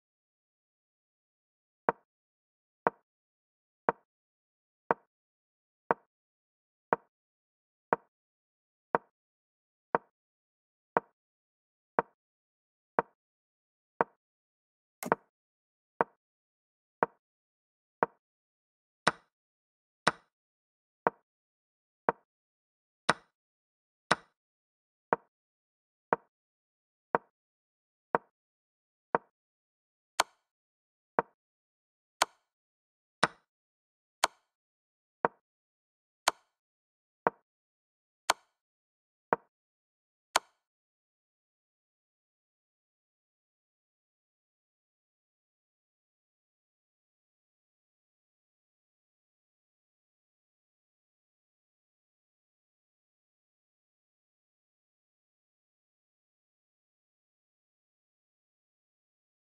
The Chinese Immortal | Liu Wenzhe vs Jan Hein Donner analyzed by Stockfish | Olympiad 1978
The Chinese Immortal is a famous match won by Liu Wenzhe. The name of the game is inspired by '' The immortal Game '' played by Adolf Anderssen in 1851

White: Liu Wenzhe (2473)
Black: Jan Hein Donner (2490)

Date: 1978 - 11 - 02

Site: Buenos Aires ARG

Event: Olympiad

Result: White won (1-0)